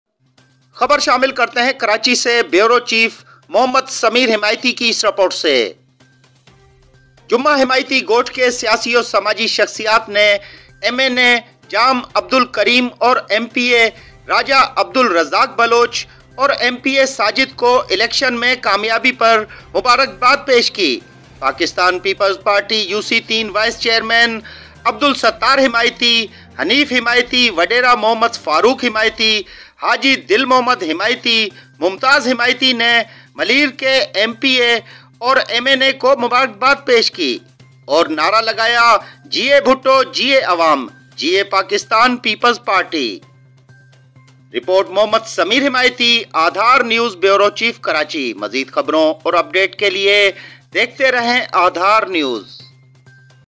Juma Hamaiti Goth congratulated the People's Party leader for winning the election
بیورو چیف کراچی محمد سمیر حمائتی جمعہ حمائتی گوٹھ کے سیاسی و سماجی شخصیات نے ایم این اے جام عبدالکریم اور ایم پی اے راجہ عبدالرزاق بلوچ اور ایم پی اے ساجد کو الیکشن جیت نے کی مبارک باد پیش کیا پاکستان پیپلز پارٹی یوسی 3 وائس چیئرمین عبدالستار حمائتی حنیف حمائتی وڈیرہ محمد فاروق حمائتی حاجی دل محمد حمائتی ممتاز حمائتی نے ملیر کے ایم پی اے اور ایم این اے کو مبارک باد پیش کیا اور نعرہ لگایا جئے بھٹو جئے عوام جئے پاکستان پیپلز پارٹی ملیر